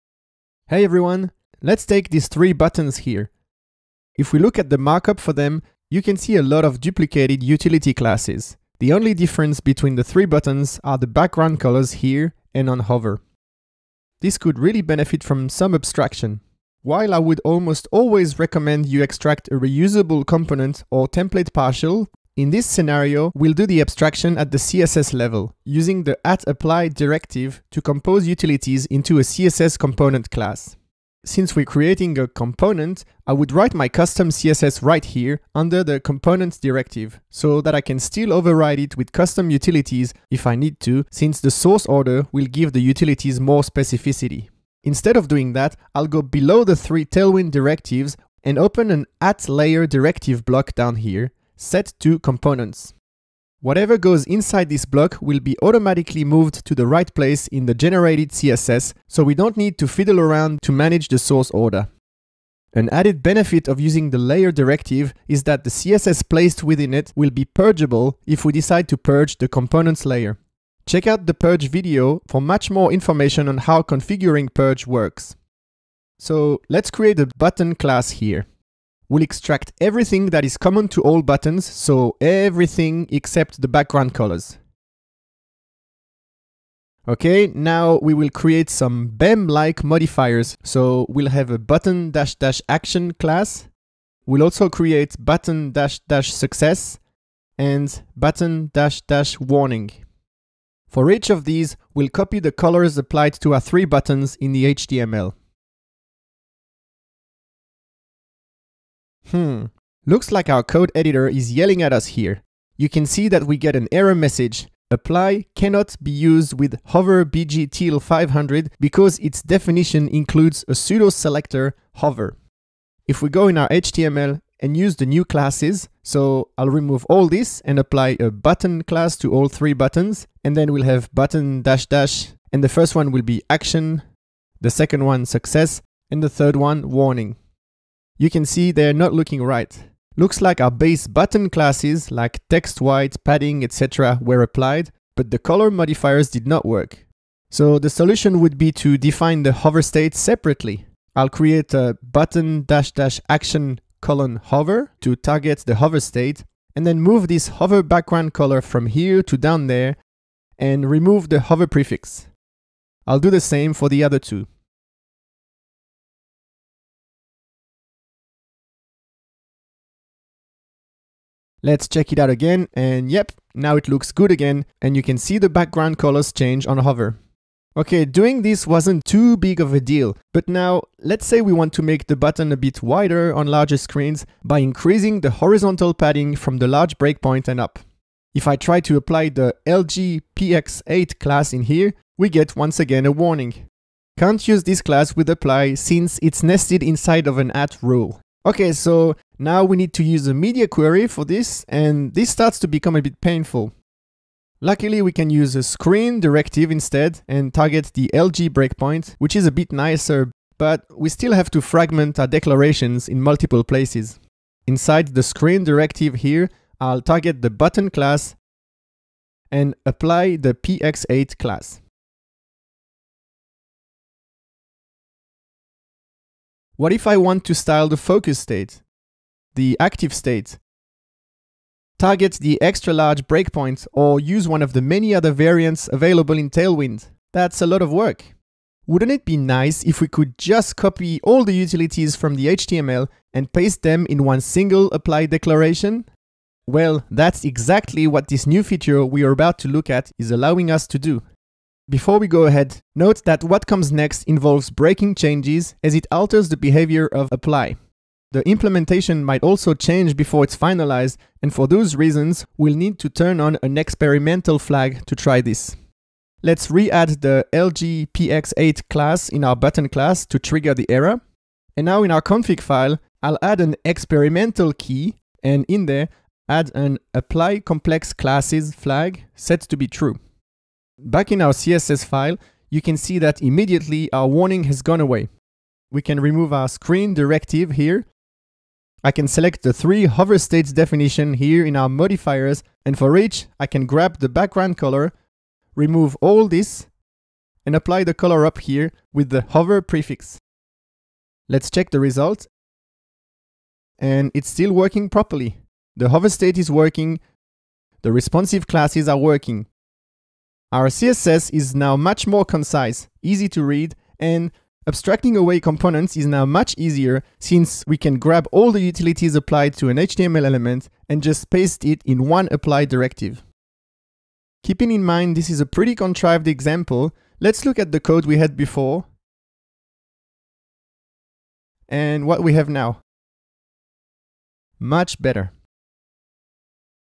Using @apply with Complex Classes – What's new in Tailwind CSS
Let's look at how the new, experimental `applyComplexClasses` feature flag added in Tailwind CSS v1.7 allows you to use any class with `@apply`, including hover variants, responsive variants, and more.